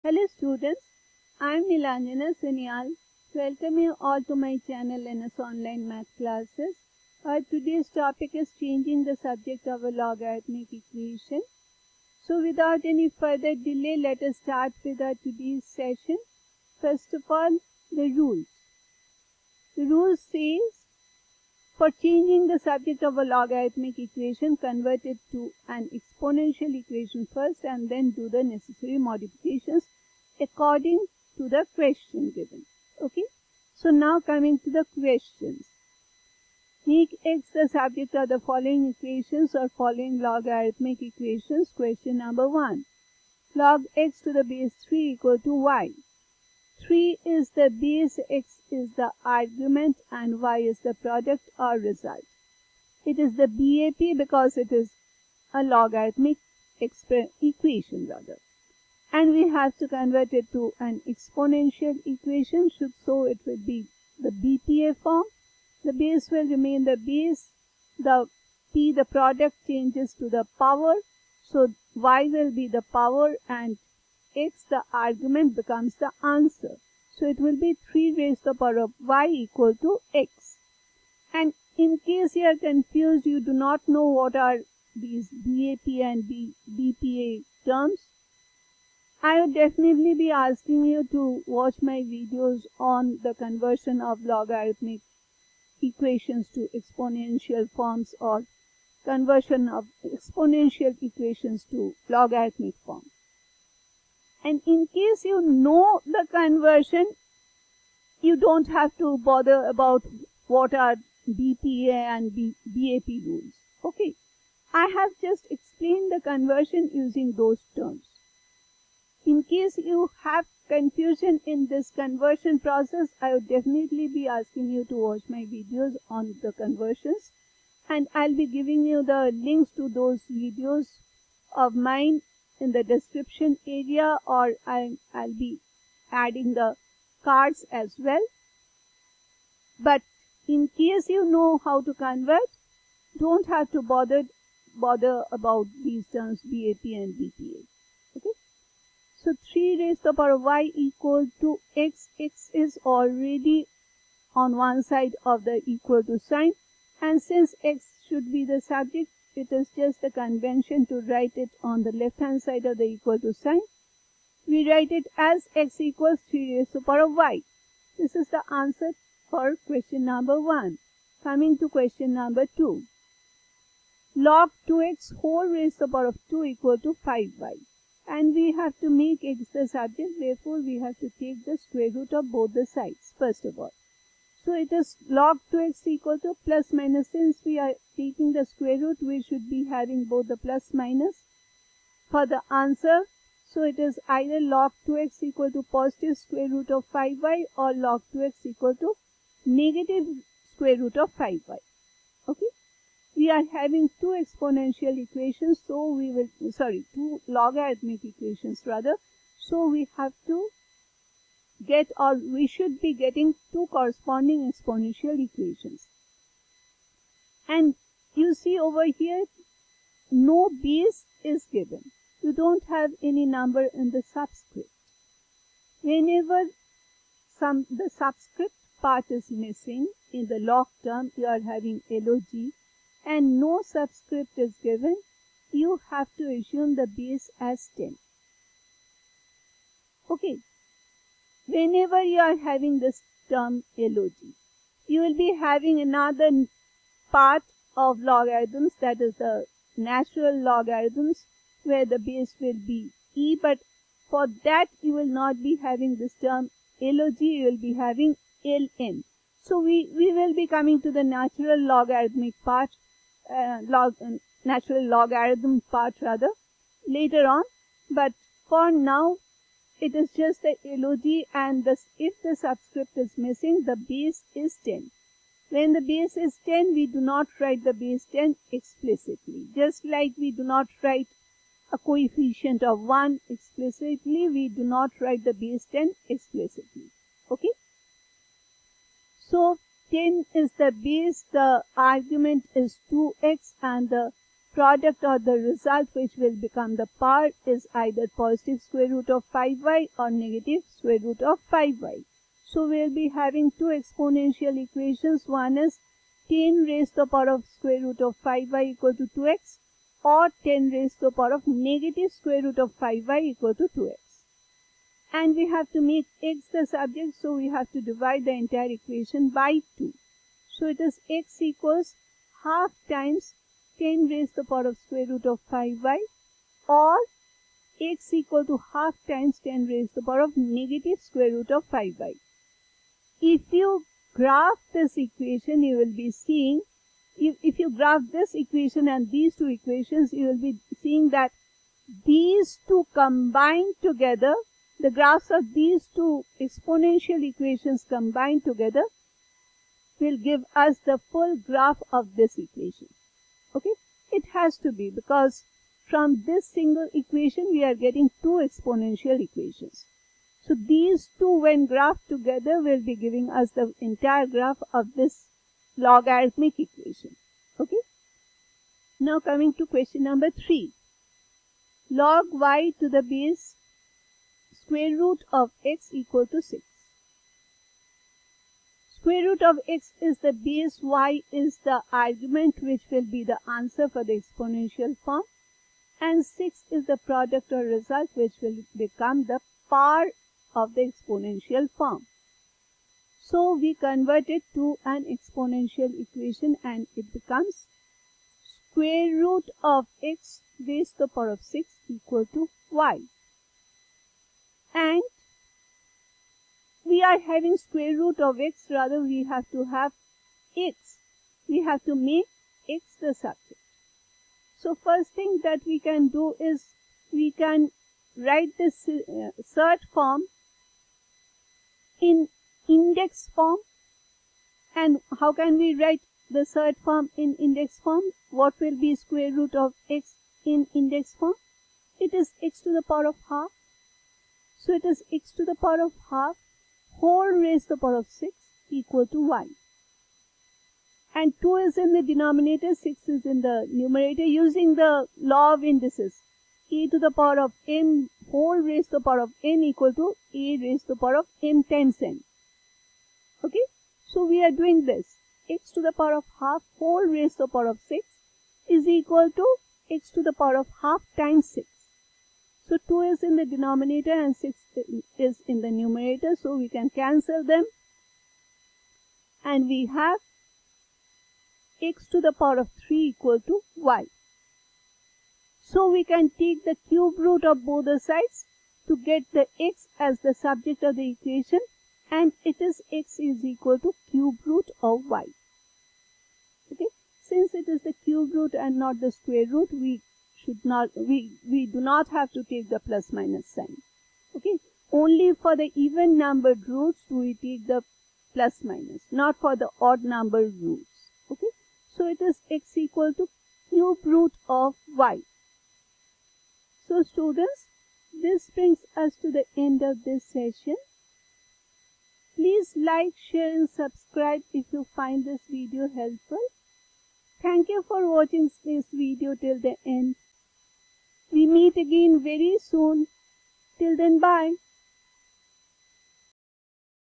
Changing The Subject Of A Logarithmic Equation
Change the subject of a log equation. How to make x the subject of a log equation? Changing the subject of a log equation using exponents.

At 0:00 minutes the topic of this video is discussed.
At 0:18 minutes the relevant rules are given.
At 0:36 minutes the first question of conversion is taken up and discussed.
At 2:53 minutes the second question of conversion is taken up and discussed.
At 6:10 minutes the third question of conversion is taken up and discussed.
 At 8:27 minutes ending of the session.

Thank you for joining me. Have a fantastic day!